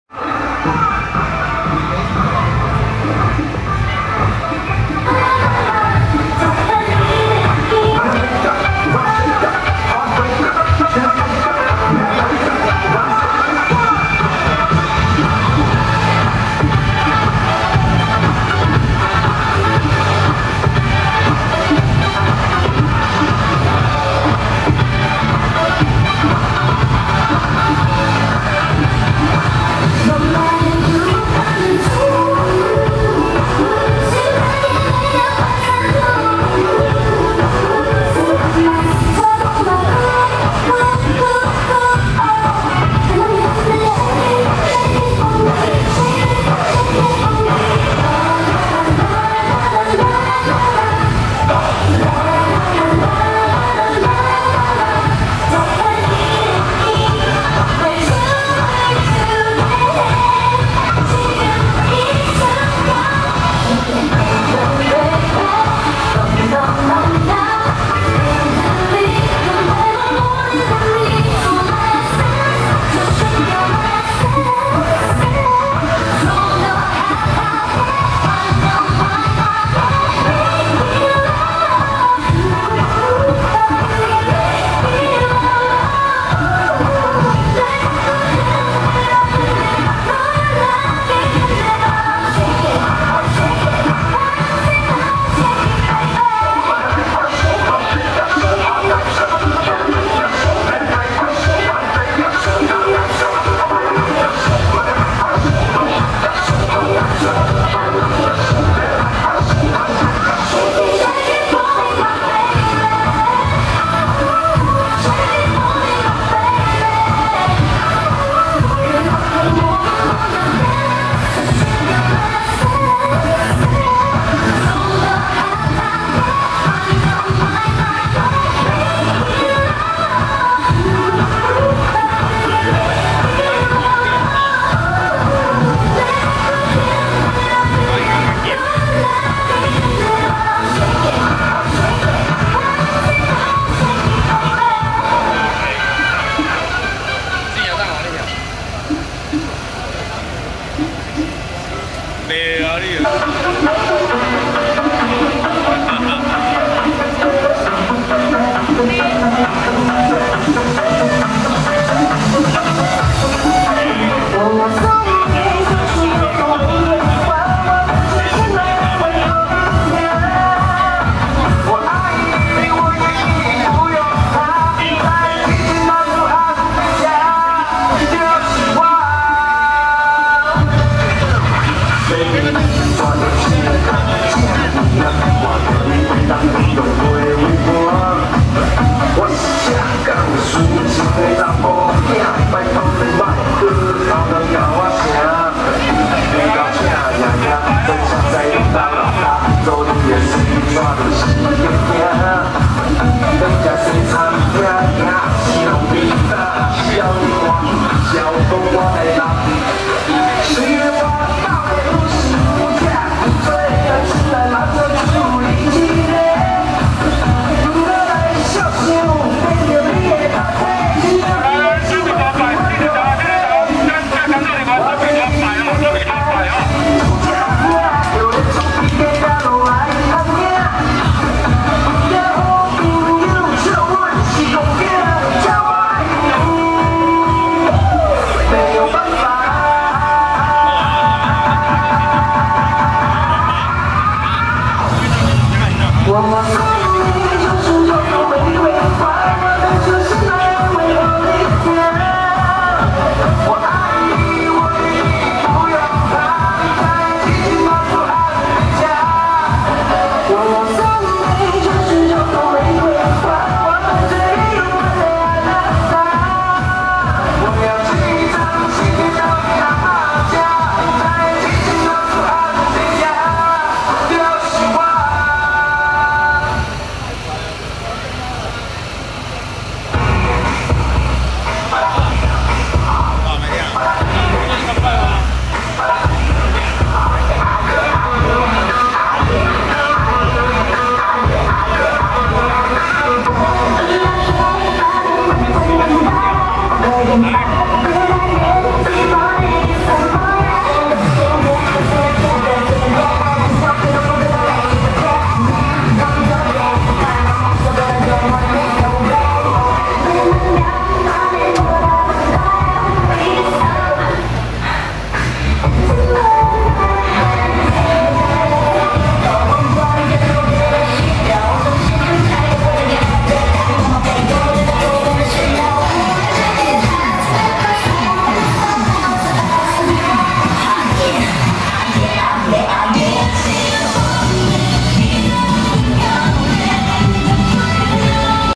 105桃園聖德堂關聖帝君三年圓科南巡謁祖進香回駕遶境【極速藝陣辣妹(多多、筑筑、凱蒂、芸兒、娣娣)】
極速藝陣辣妹(多多、筑筑、凱蒂、芸兒、娣娣)